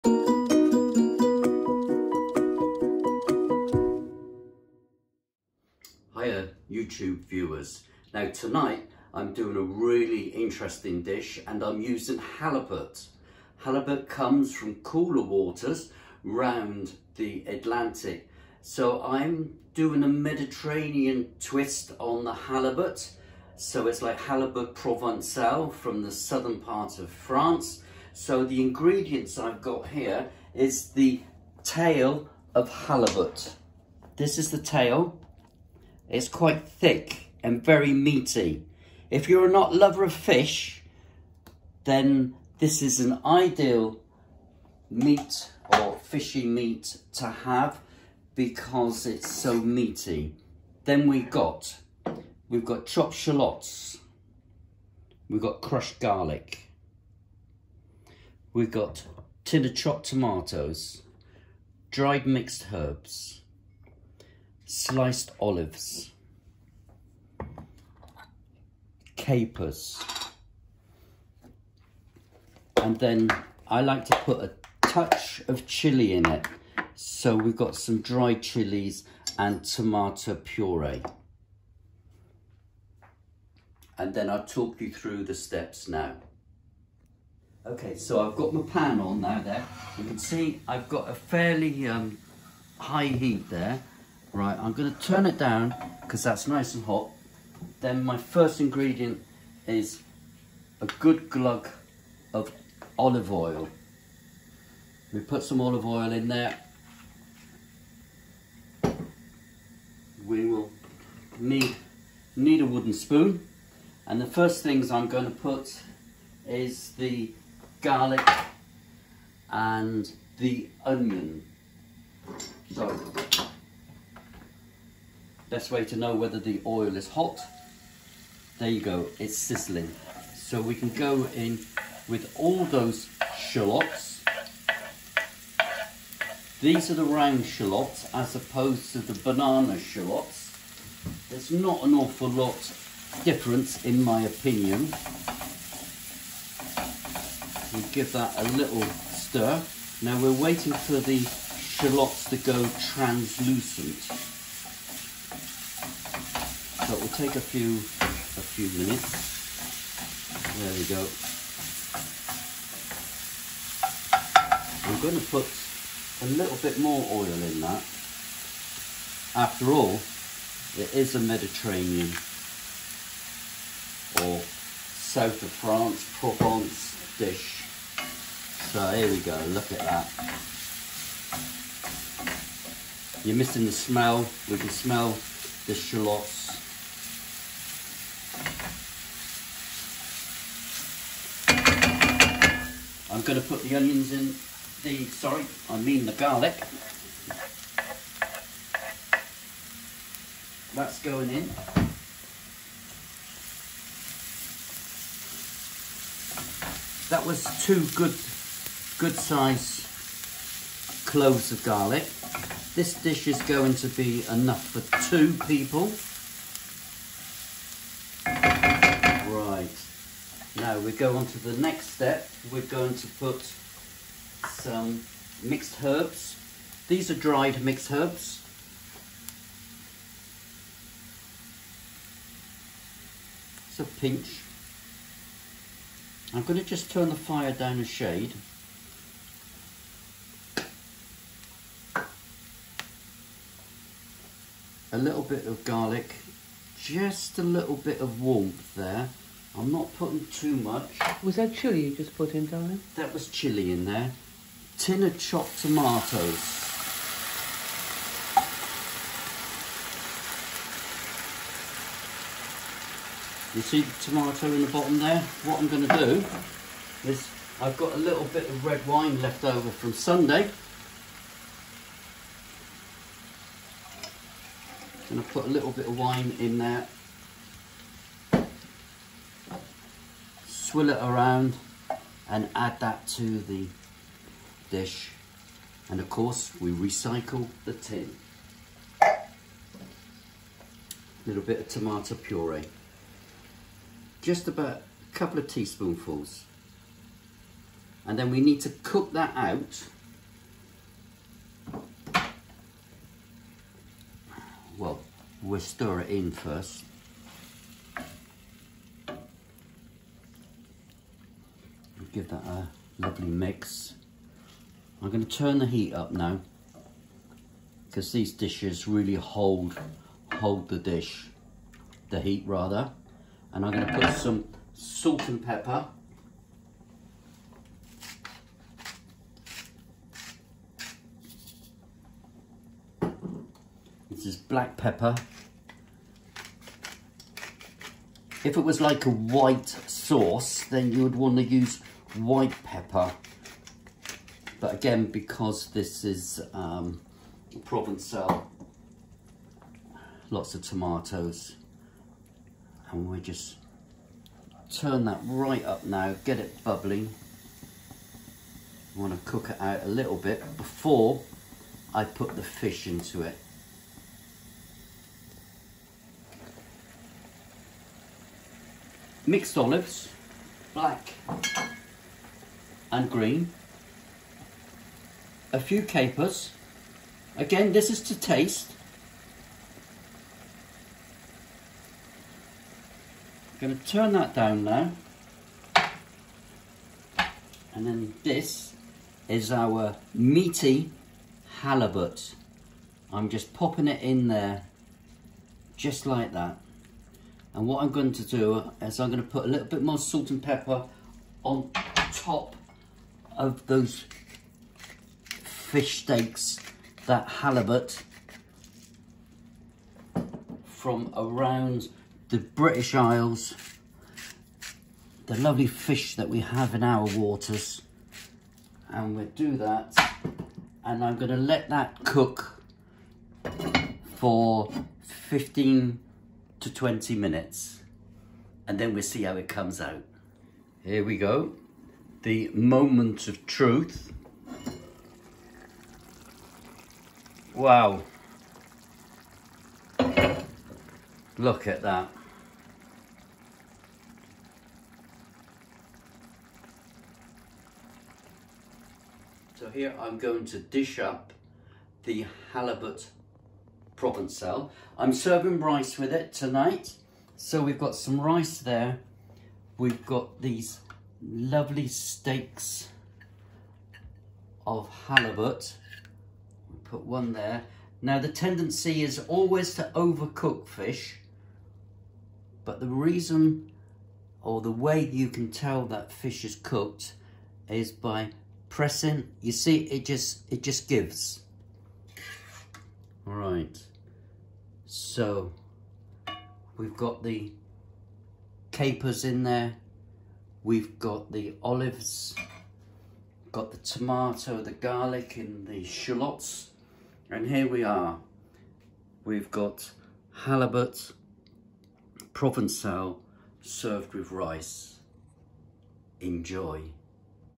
Halibut Provencal
A very appetising fish dish using tail of halibut, fished from the waters around the UK, combined with a rich tomato sauce in the style of province region of France.